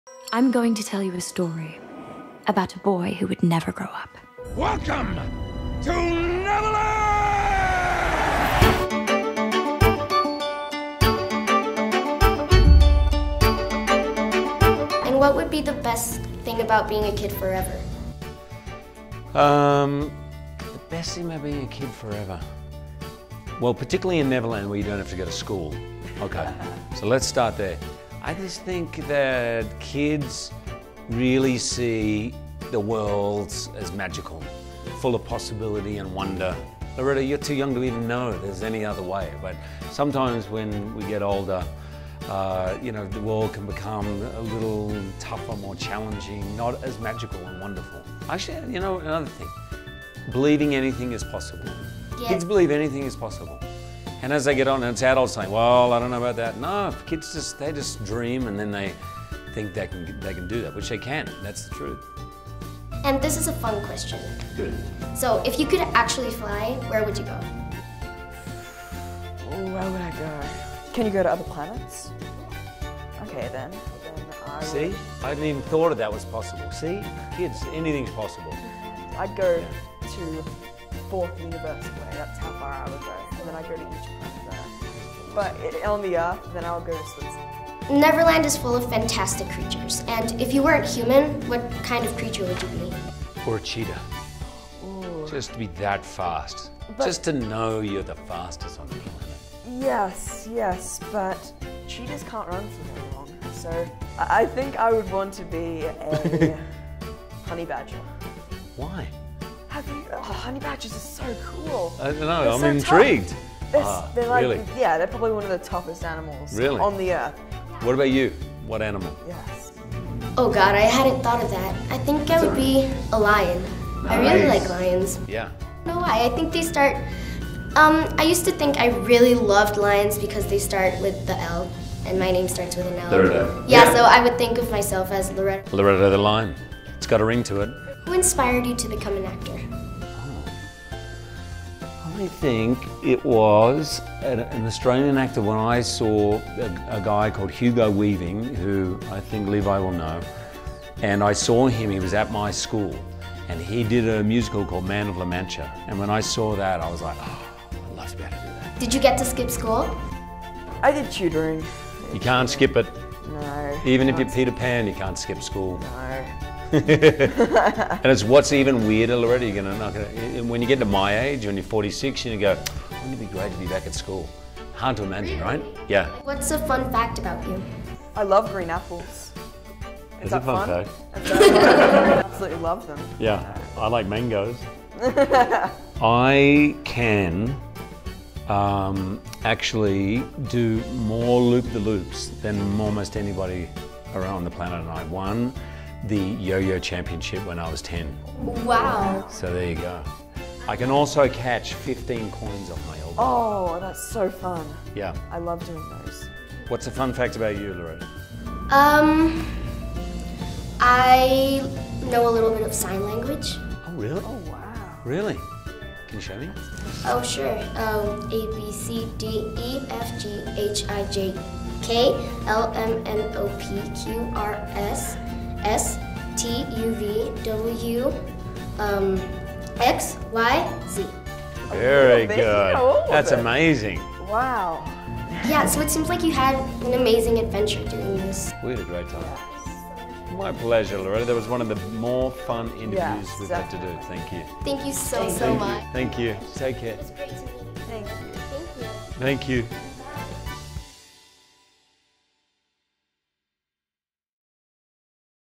Loreto Peralta le hizo la entrevista más tierna a Hugh Jackman
La súper estrella más joven de México entrevistó a una de las estrellas más grandes de Hollywood por el estreno de la película Peter Pan en México